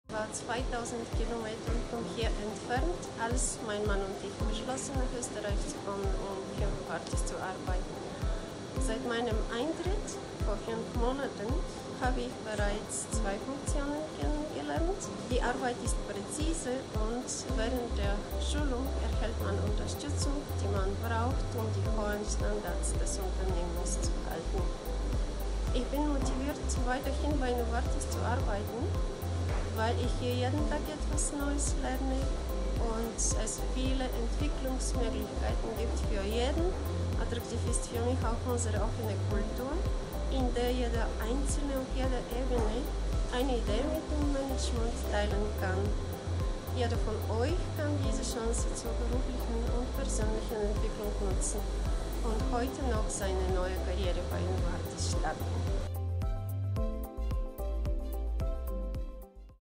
Meet Tania, Plant Operator on career opportunities with Novartis
Novartis Careers: careers with purpose